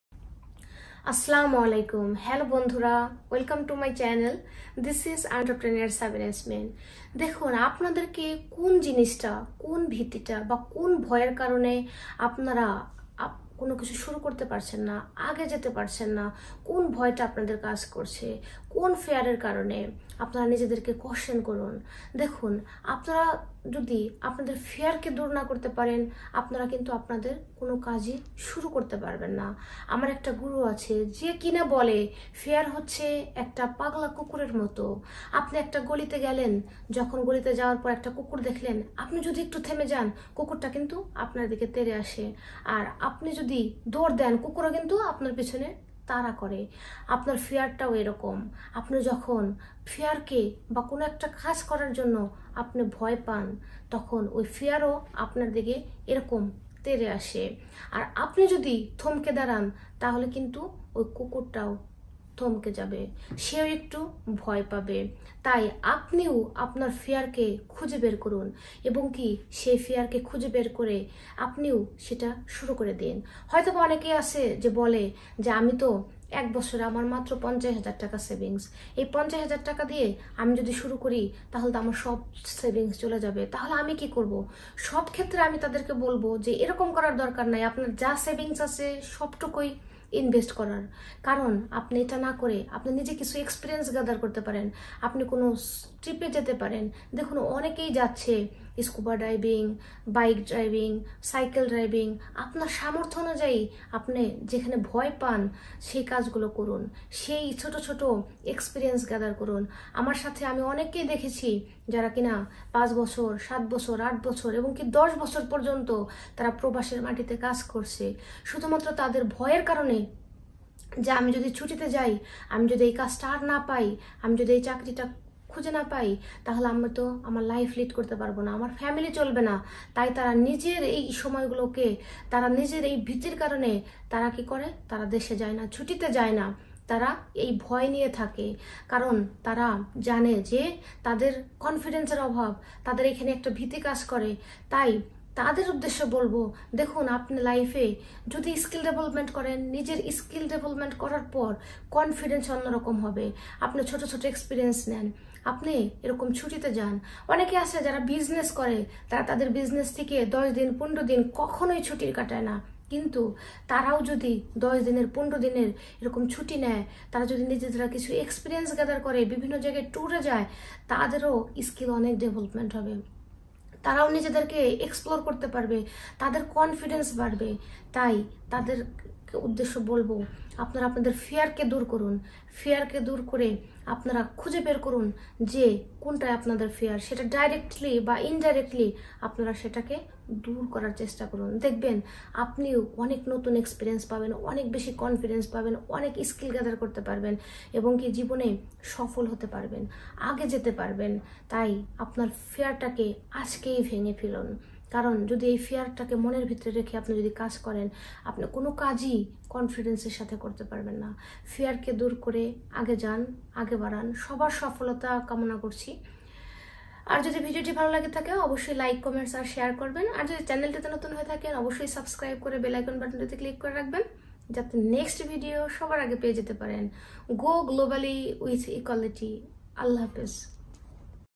How to Overcome Fear? ভয়কে জয় করুন || by Entrepreneur Sabina Yasmin
Hi this is Entrepreneur Sabina Yasmin in this video you will learn how to overcome fear and success in life. If you are still could not start anything because of fear watch this video until end and get some experience.